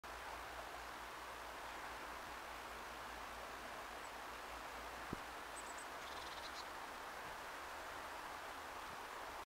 Blue tit and Aphids
Blue tit (Cyanistes caeruleus) throughly collected Aphids on a small tree on the bank of the river. In five minutes a breakfast there was finished and the bird flew to the bushes closer to the water.
Bank of Nioman river inside Hrodna city, W Belarus, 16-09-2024